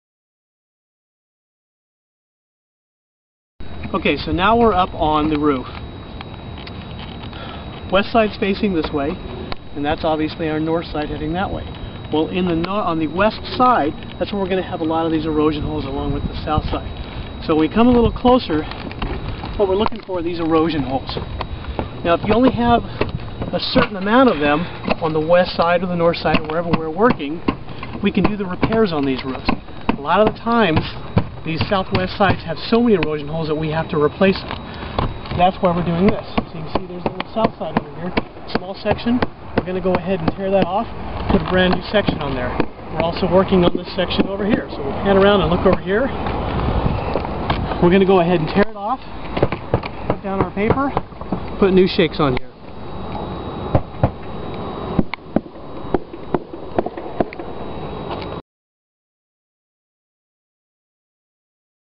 Partial Roof Replacement 2 - Roof Life of Oregon
- Daniel Whites shows us the West side of the roof where we expect to see some visible wear and tear.